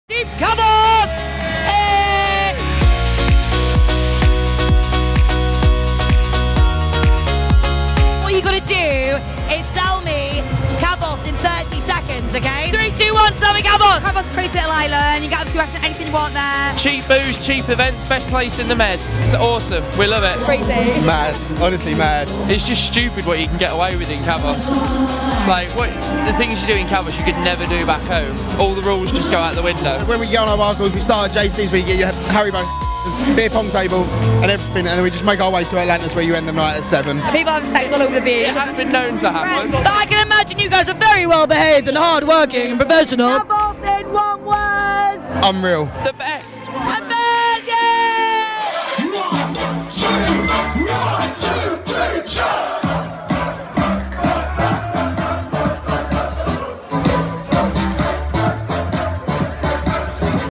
Kavos 2016: A Rep's Guide To Kavos
Tempted to book a Club 18-30 holiday to Kavos?

Well, here is everything you need to know about one of the hottest party destinations in the world.